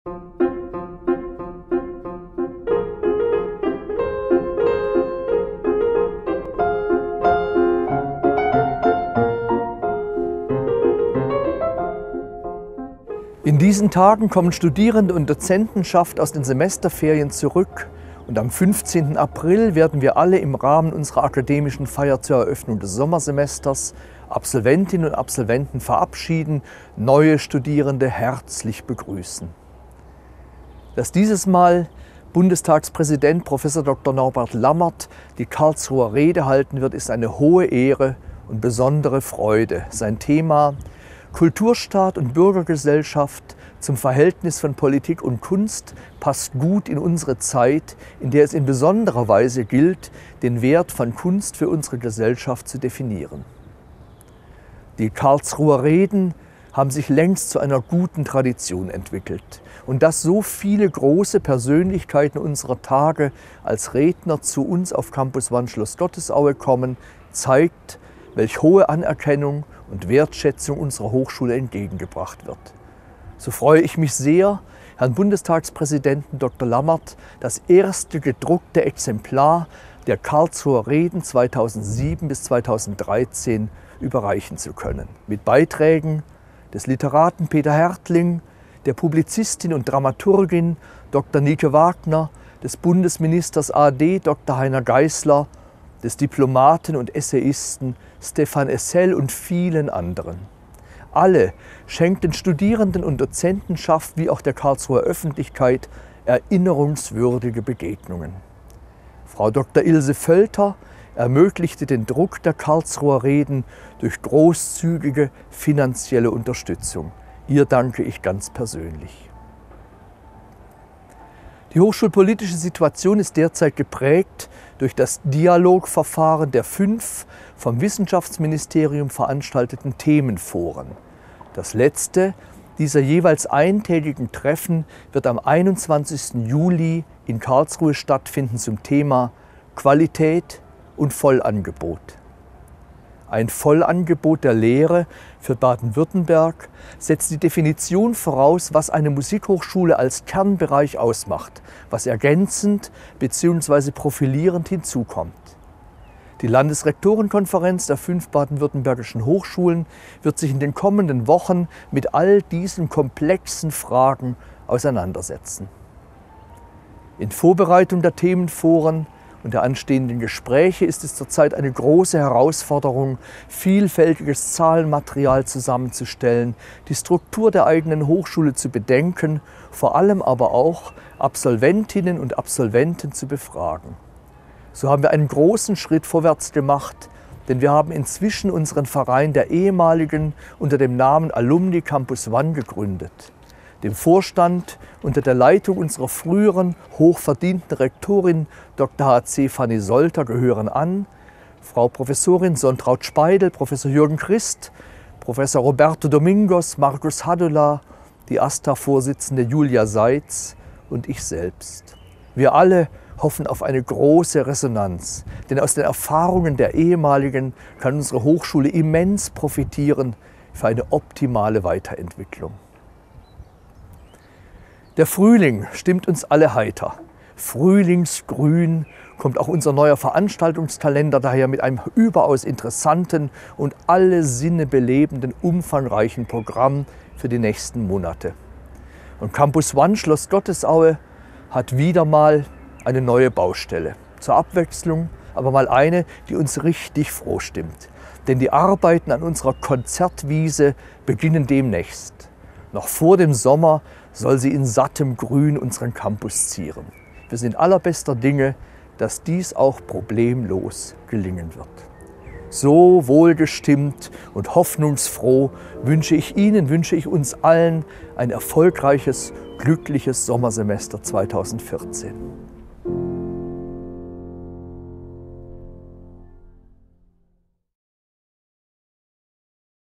Podcast des Rektors, März 2014 - Hochschule für Musik Karlsruhe
In der März-Ausgabe freut sich Hartmut Höll auf die nächste Karlsruher Rede, welche von Bundestagspräsident Professor Dr. Norbert Lammert gehalten wird. Außerdem gibt er erste Informationen zum neu gegründeten Alumni-Verein "AlumniCampusOne" und zur Entstehung der Konzertwiese auf CampusOne - Schloss Gottesaue.